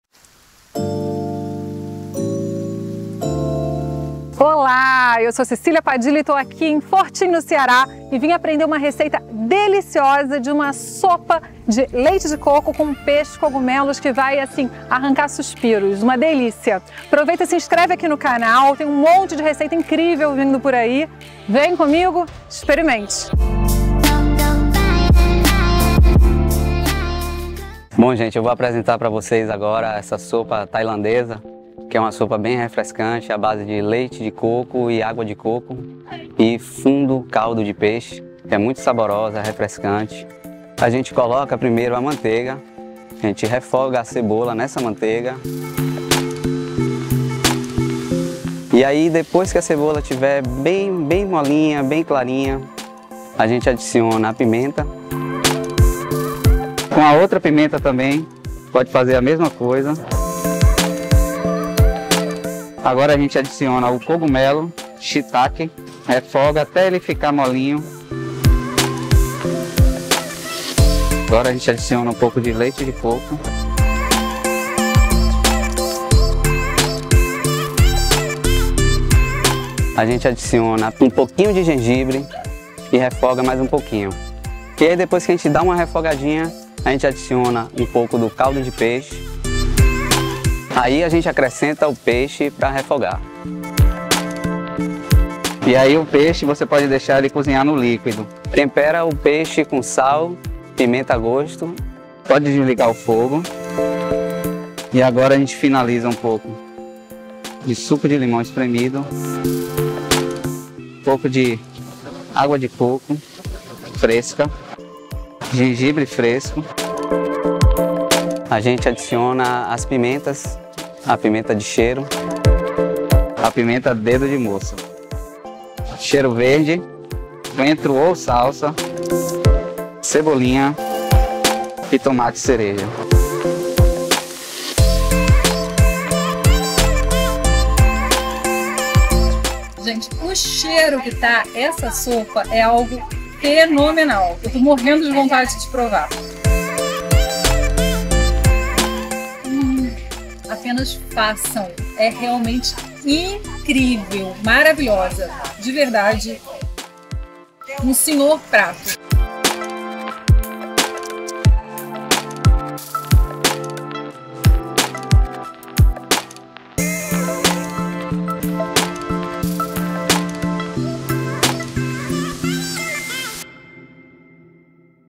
Receita de sopa thai com leite de coco (Maravilhosa para inverno)
Receita imperdível nesse frio. Além de deliciosa é também perfeita para os dias de frio. O Chef Marcel Coura do Hotel Vila Selvagem em Fortim no Ceará
A sopa é surpreendente e vale por uma refeição inteira. Super fácil de fazer e sucesso garantido.

Ingredientes:

200g de peixe
100g de shitake
2 xícaras de leite de coco
1 xícara de água de coco
150ml de caldo de peixe aromatizado com capim limão e gengibre
1 cebola
5 tomates cereja
3 pimentas de cheiro
1 dedo de moça
Meio limão siciliano
Meio suco de laranja
Coentro ou salsa a gosto
Azeite de oliva a gosto